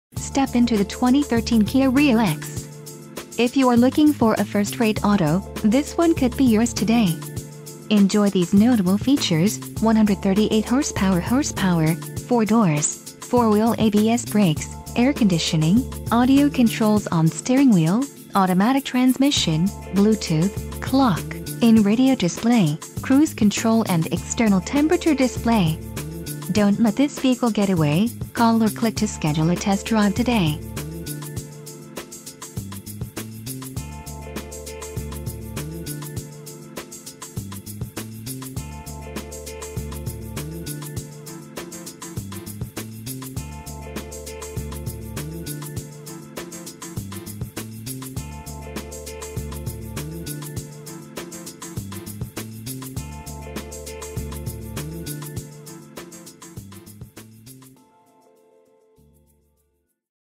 2013 Kia Rio EX - Orlando KIA West - Orlando, FL 32808
KIA Certified **10yr/100K Mile Warranty**, Balance of Factory Warranty, Clean AUTOCHECK no Accidents, Full Safety Inspection, and One Owner. How much gas are you going to start saving once you are riding away in this outstanding 2013 Kia Rio? Why take the bus, when this can get you there so much more conveniently and comfortably? Such fantastic gas mileage that you'll spend about as much on gas as you would have the bus fare!